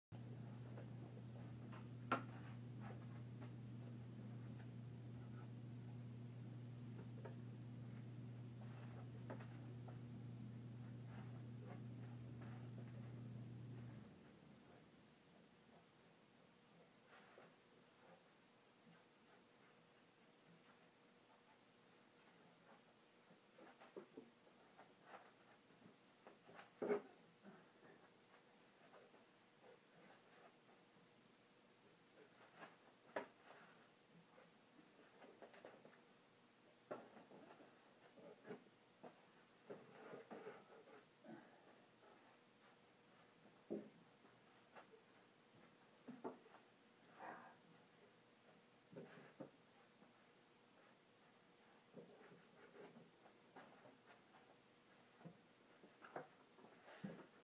Artist Jeremy Adams paints layers on painting
Hippie style painting for Humble Pie